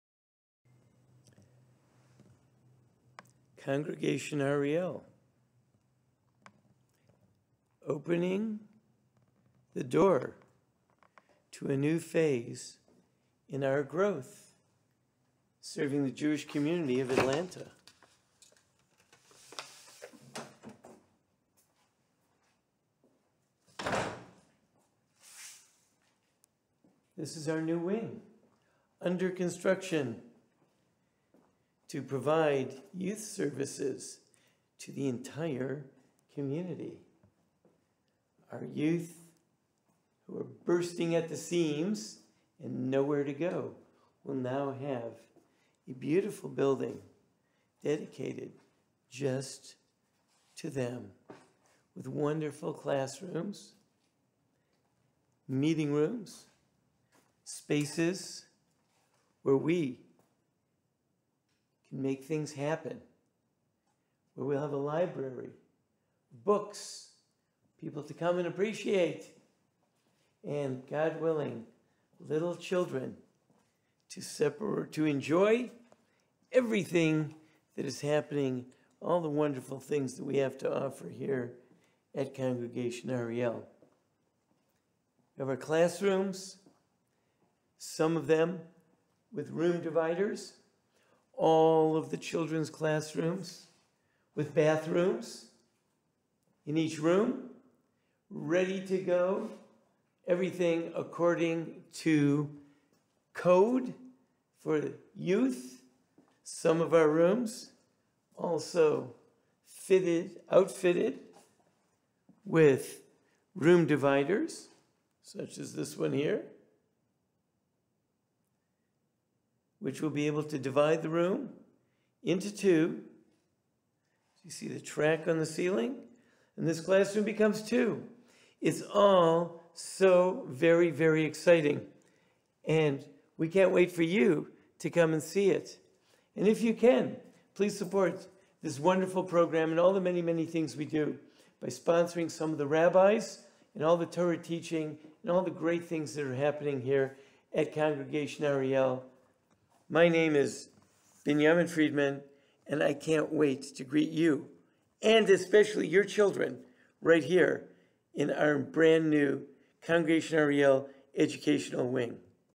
New wing
Rabbi Friedman tours the new wing.